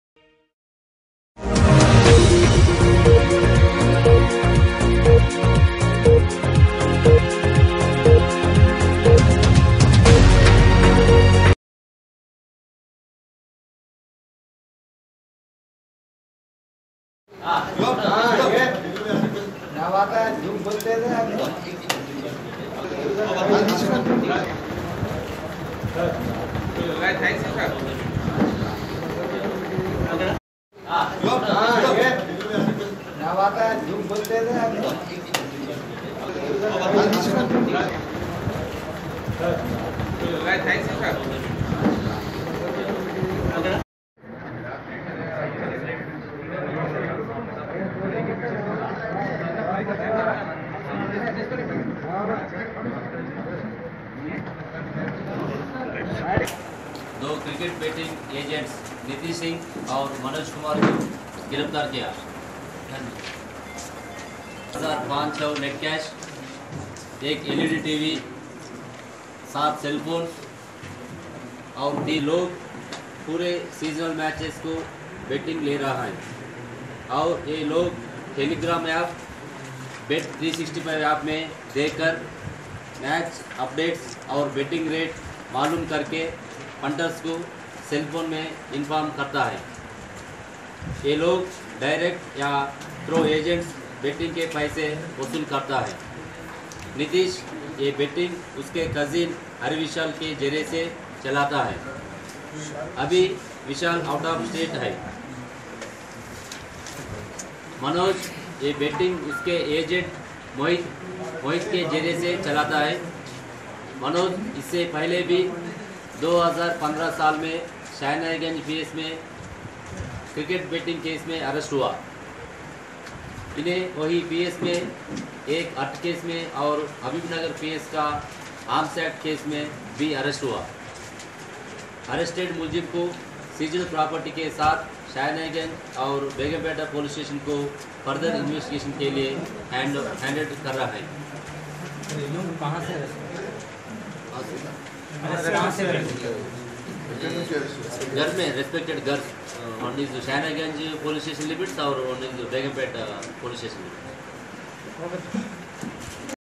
CITY tv NEWS.ADDL DCP TASK FORCE N.KOTI REDDY TWO CRICKET BETTING.TWO HELD SEIZED 7,01,500 ONE LED
CITY tv NEWS.ADDL DCP TASK FORCE N.KOTI REDDY TWO CRICKET BETTING.TWO HELD SEIZED 7,01,500 ONE LED 7 cell PHONES